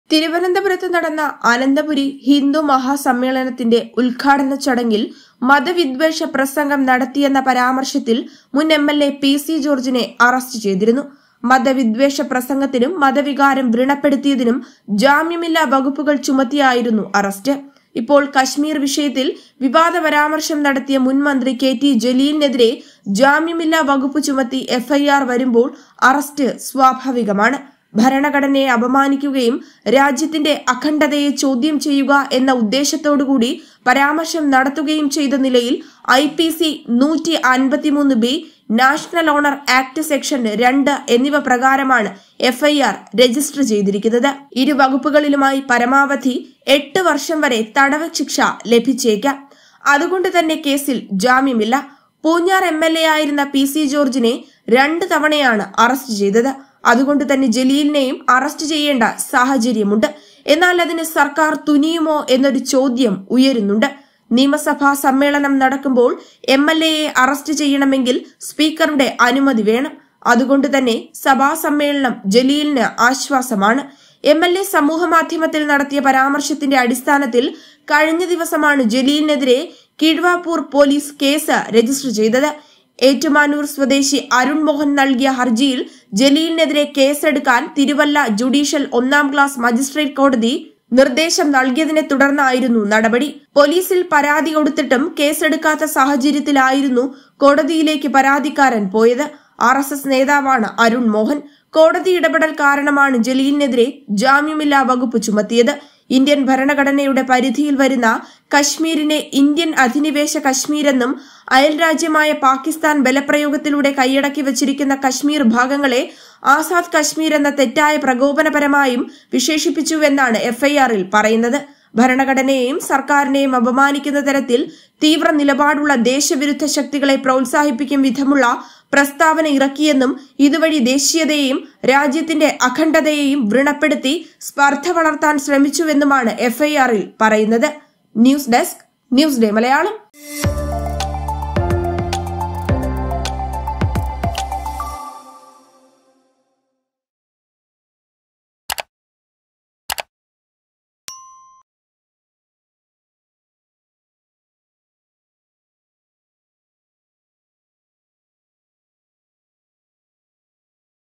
അറസ്റ്റ് ഒഴിവാക്കാന്‍ കഴിയില്ല! | KT JALEEL | NEWS DAY MALAYALAM
ജലീല്‍ ജയിലില്‍ പോകും!

അറസ്റ്റ് ഒഴിവാക്കാന്‍ കഴിയില്ല!